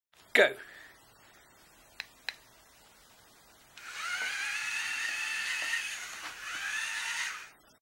Orion Explorer One Pushing a Ball
is following a simple move and turn program here with my 3 yr old pushing the go button. She rather likes it.

The Arduino here is being powered by the voltage reg in the motor controller board below. With the link above - you can also buy and build a robot like this.